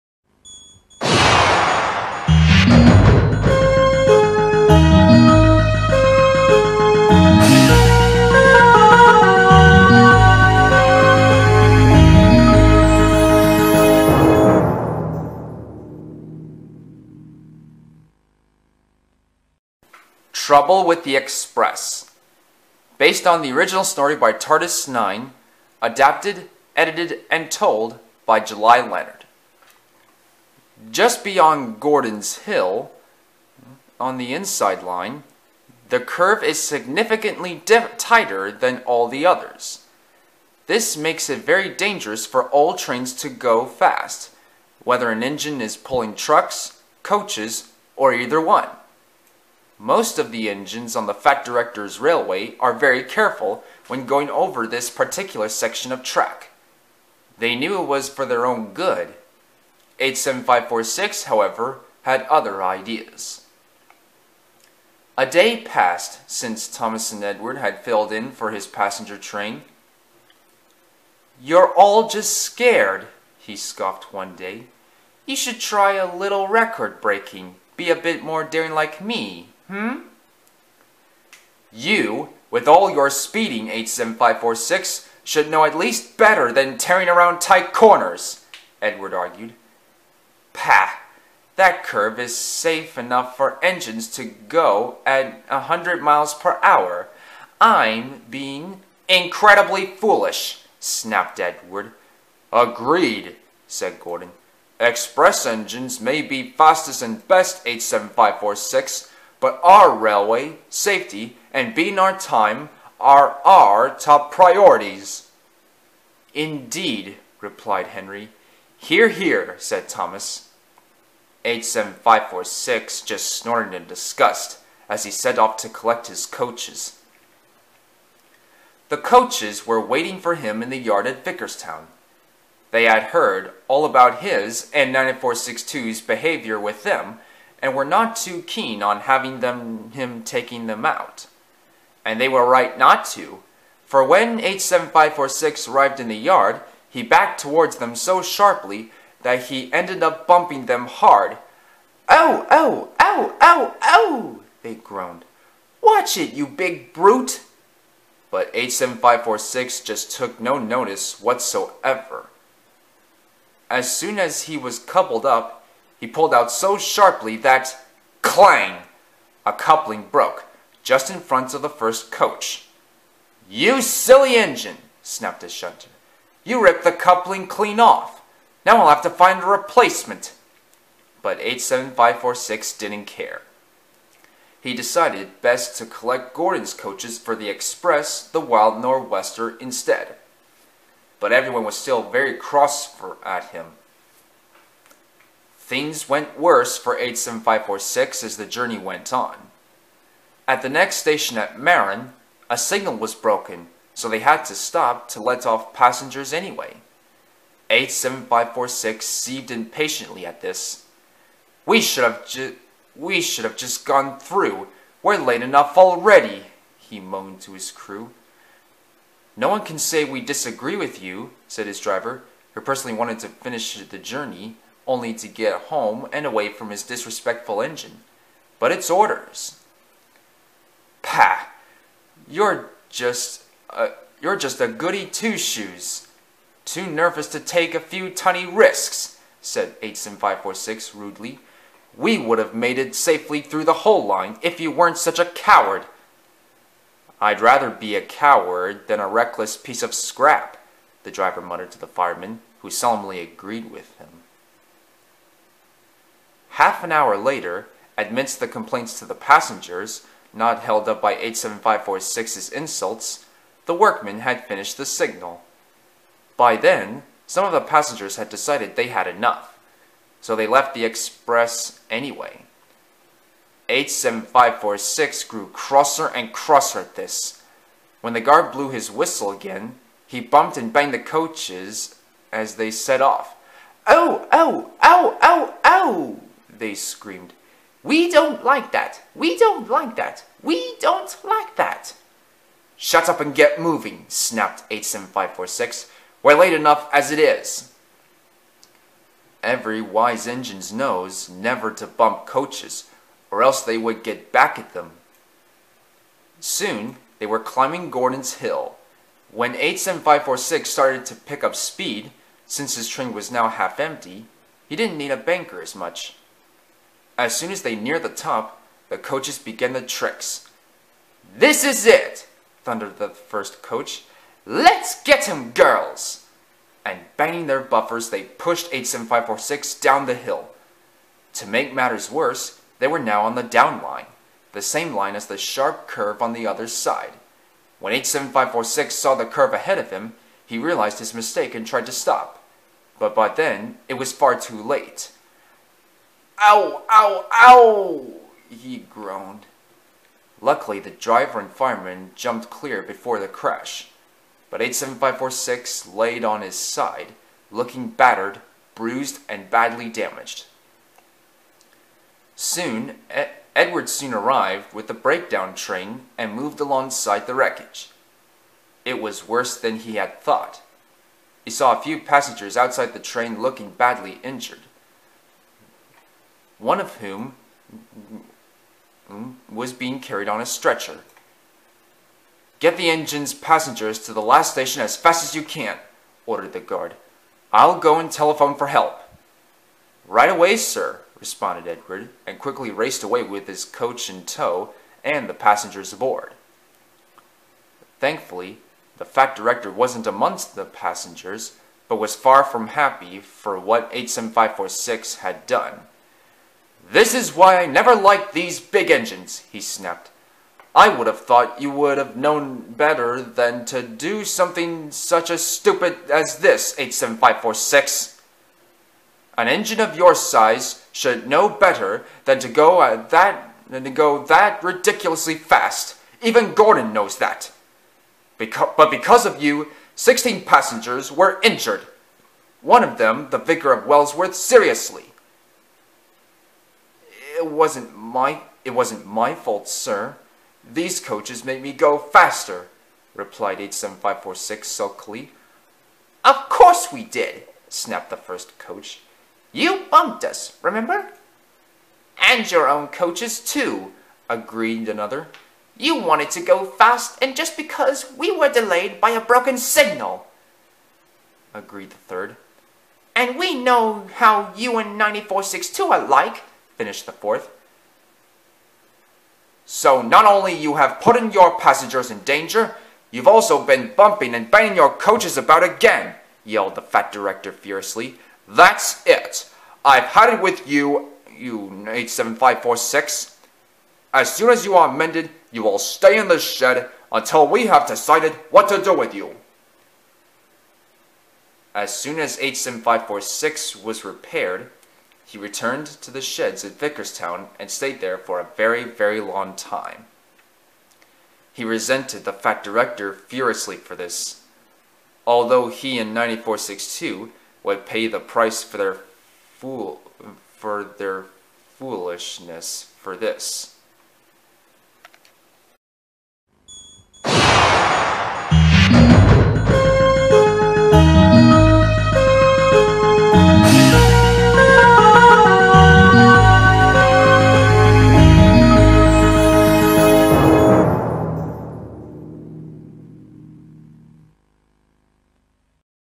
TLRWS No 1 The Loaned Engines Part 3 Trouble with The Express
For @andrewsmith6563, @Trainboy55, @carson08022000,@NewController01,@patrickward-yj4lx, @seangannon408, @NWR1991, @JamesFan1991,@Zerneygreensaberman0728, @TheBuriedTruck & many more. Hello everybody & welcome to my very first volume of my new series, The Lost Railway series, The Loaned Engines. This volume centers around two blue engines numbered 87546 & 98462 from The Railway series book No. 1, The Three Railway engines. The third story of the volume, Trouble with The Express, focuses on 87546 again, as this was based off of @TARDIS9 & @seangannon408's versions. But let me know what you think!
0:00 Intro
0:30 Story
8:36 Outro
Thomas & friends belong to Hit entertainment/Mattel.
The Railway series belongs to Egmont books.
Trouble with The Express belongs to @TARDIS9.
No copyright infringment needed or intended.